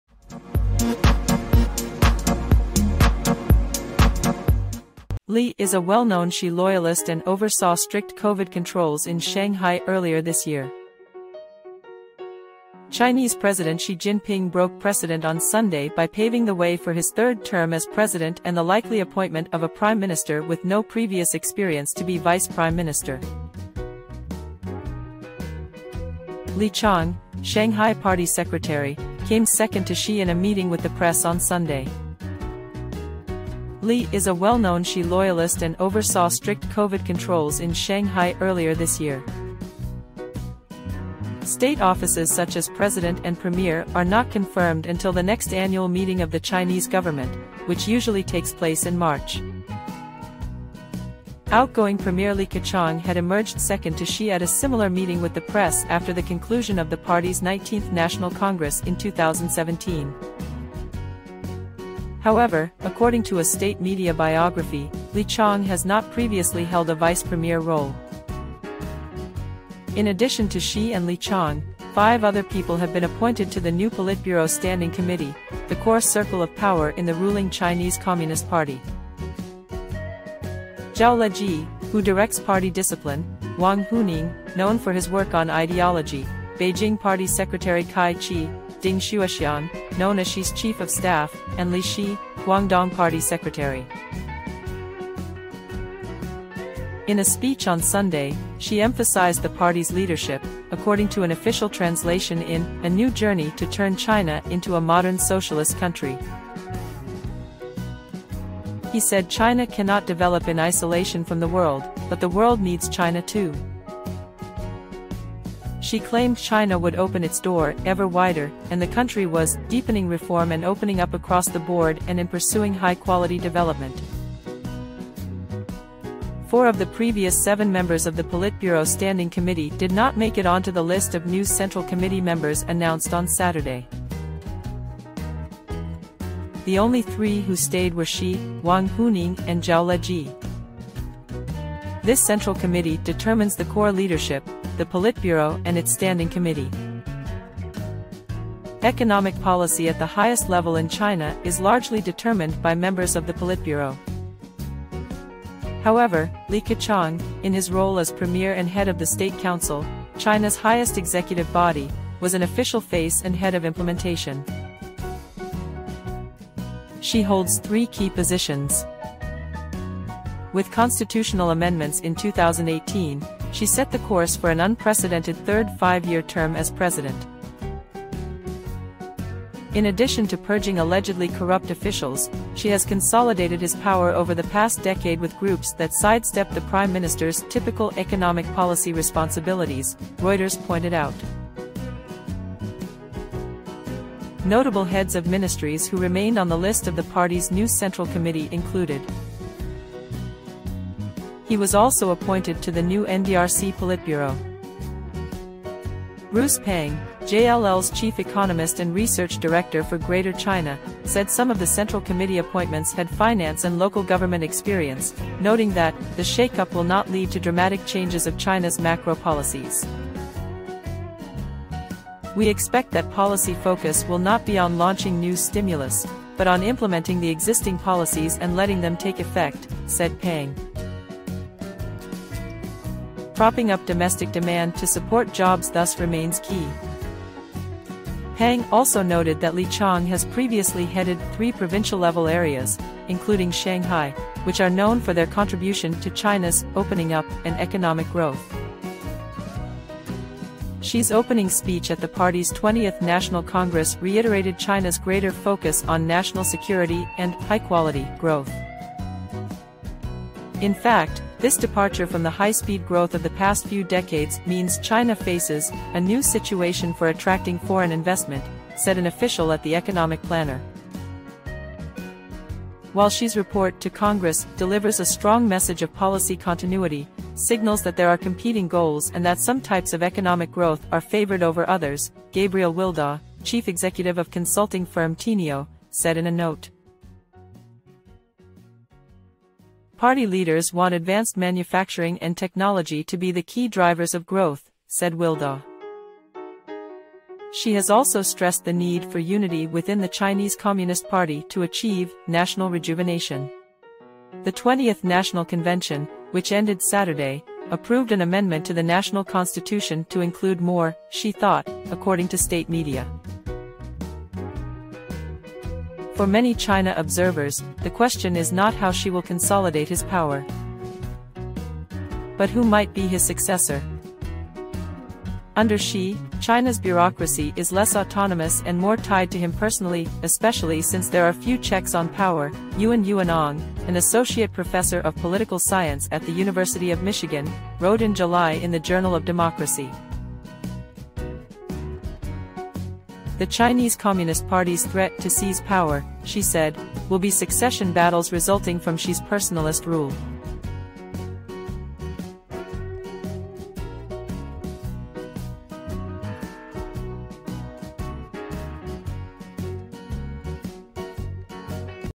China names Xi Jinping loyalists for core leadership group
Li Qiang,Xi Jinping